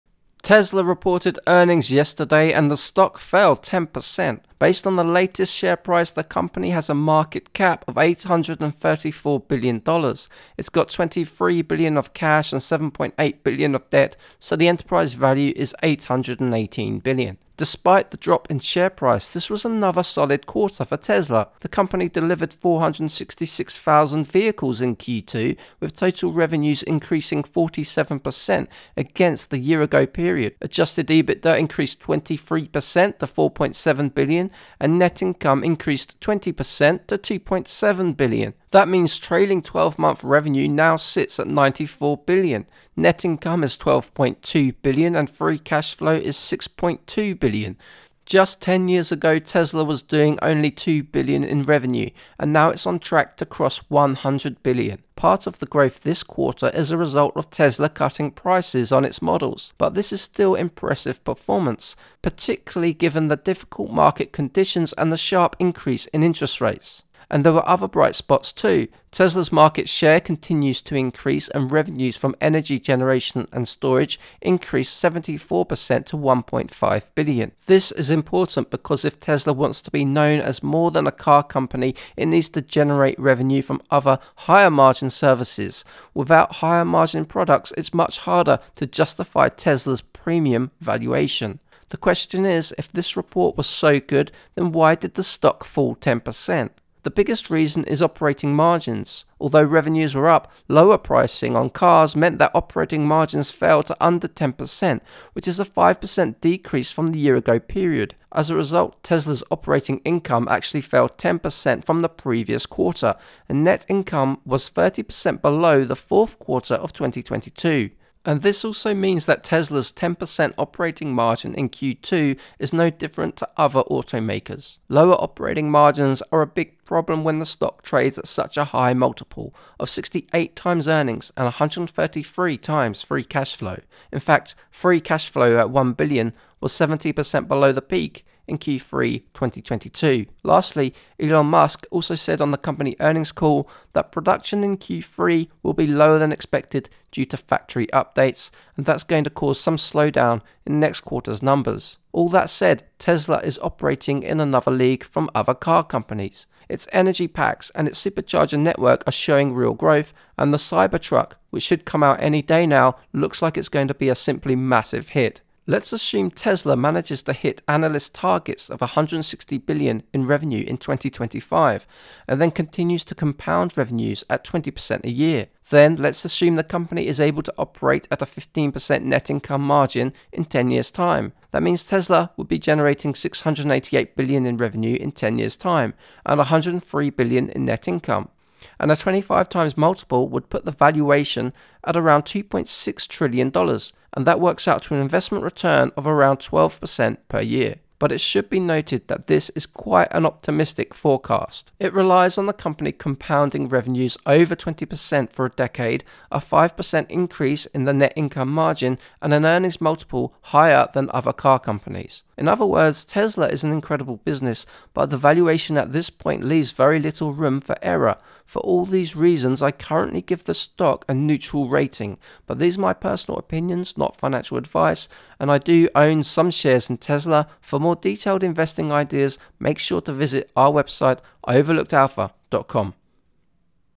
Tesla Is Still Showing Incredible Growth
Tesla stock analysis, July 2023.

Tesla famously doesn’t spend on advertising, so their operating expenses are below 10% of revenue (R&D and SG&A). Despite what might seem like a high valuation, there’s a chance that Tesla does extremely well from here onwards. They are still ramping up manufacturing capacity, which will lead to future growth, their network with 45,000+ superchargers can be used to earn fees from other car manufacturers, the energy generation and storage segment is still in its early phase, and there is a lot of hype and hope around FSD (Full self-driving) capability.